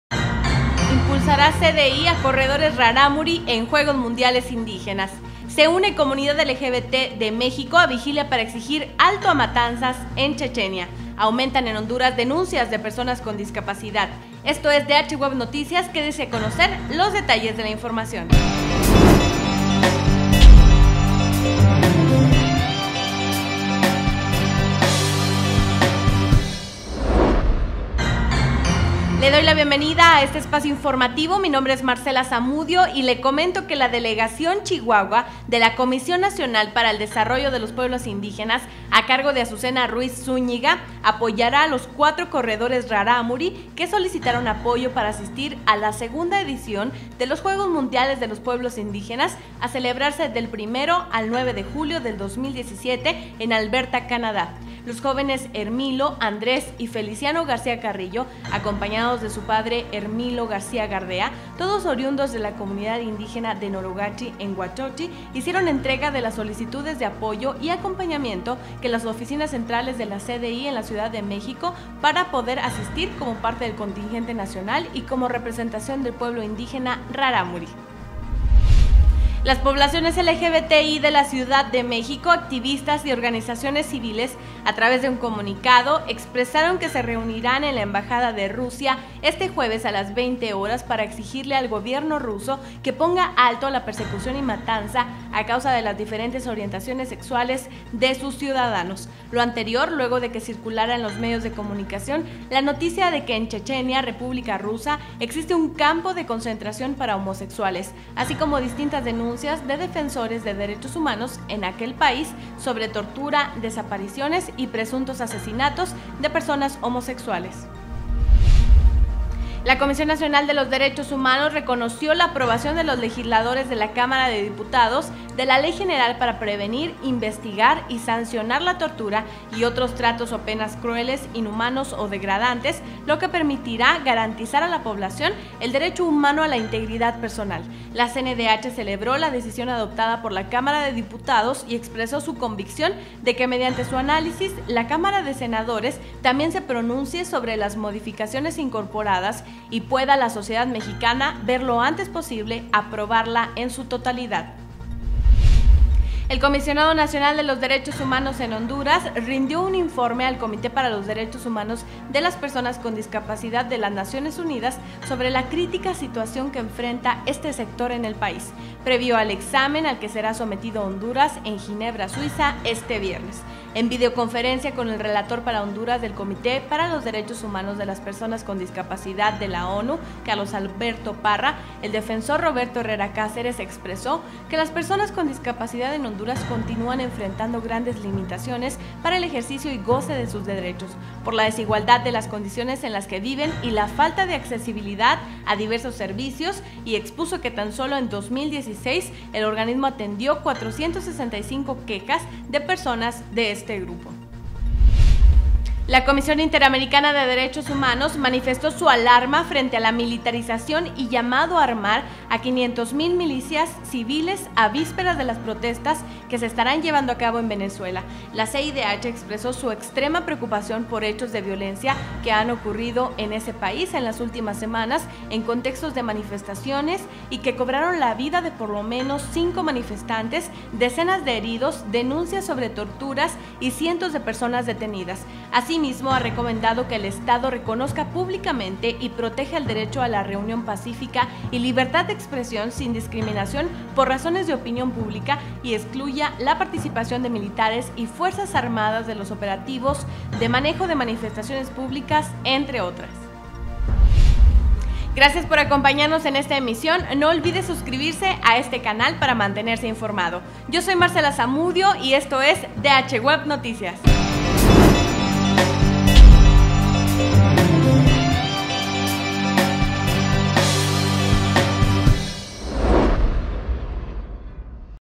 Impulsará CDI a corredores Rarámuri Juegos Mundiales Indígenas
-Se une comunidad LGBT de México a vigilia para exigir alto a matanzas en Chechenia

-Reconoció CNDH compromiso de legisladores por aprobación de Ley Contra Tortura

-Aumentan en Honduras denuncias de personas con discapacidad

-Exhortó CIDH a Venezuela para que desmilitarice las calles